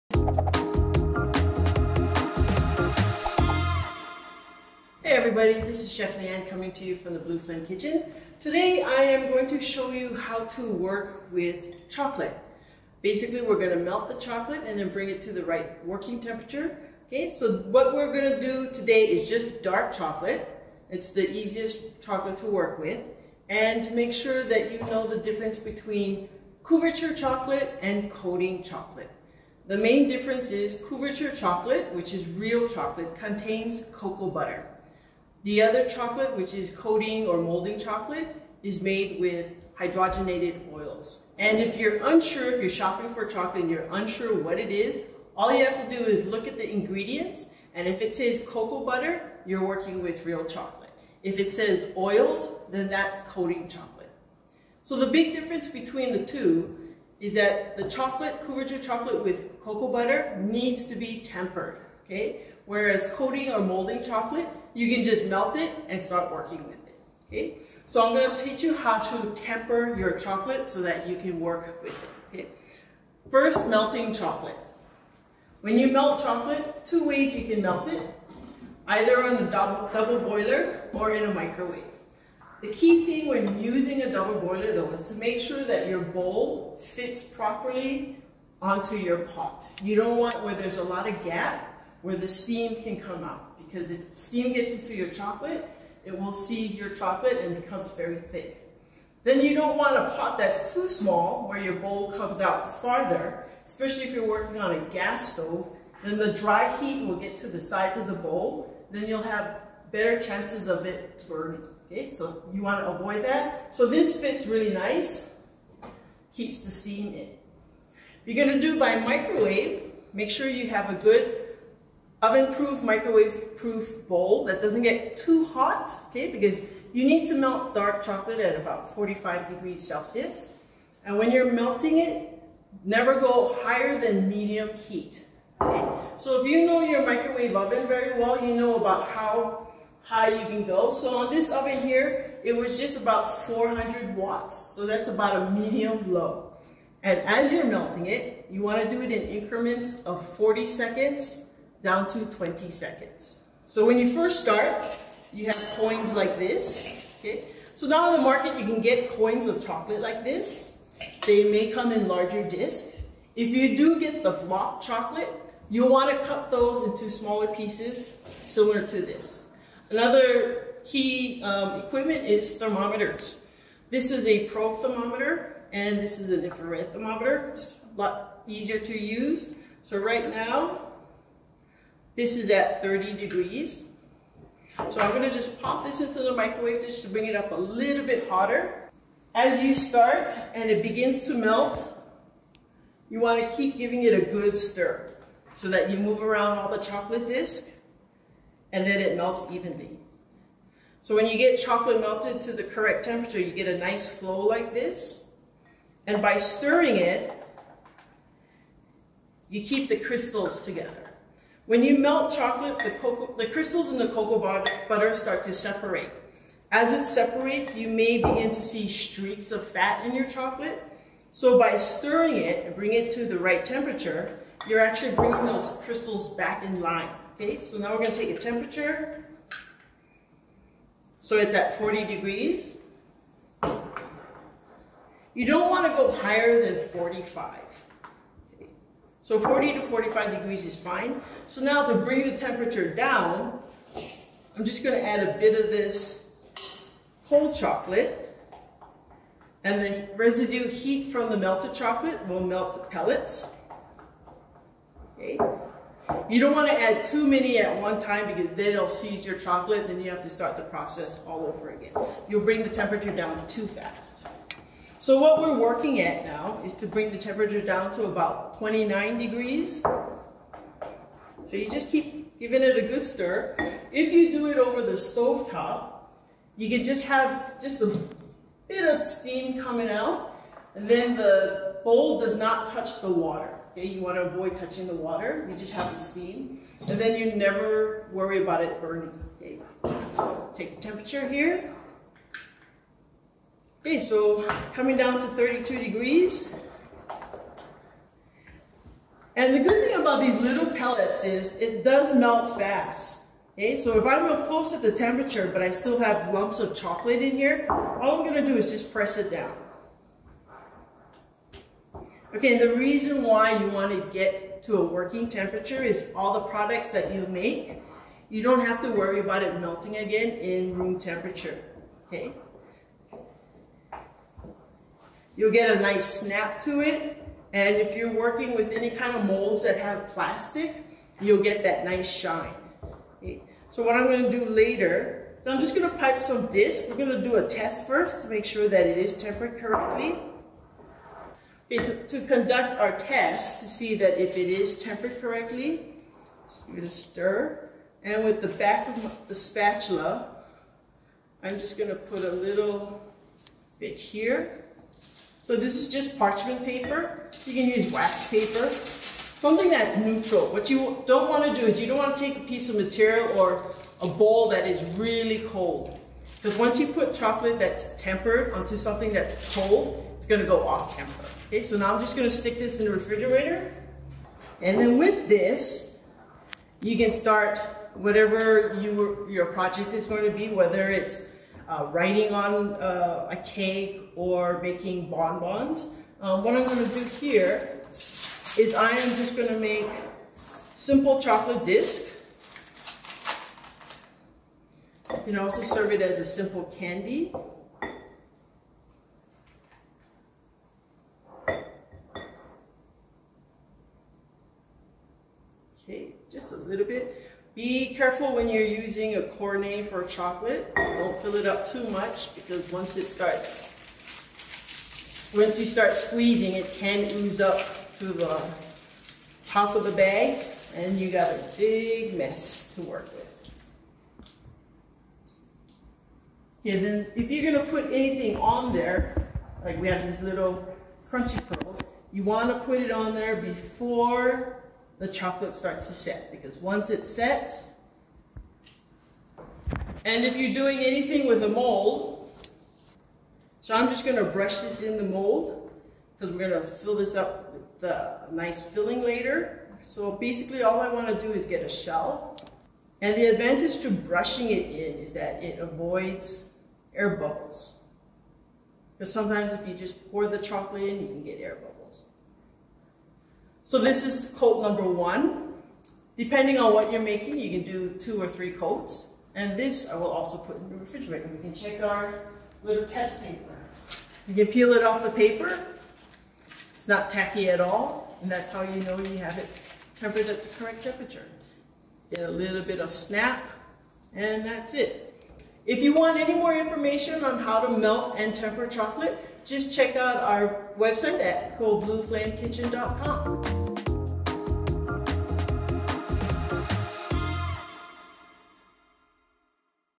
How to Temper Chocolate with Chef Leigh Anne
Blue Flame Kitchen chef and professionally trained chocolatier, Chef Leigh Anne offers some tips and tricks on how to properly temper chocolate.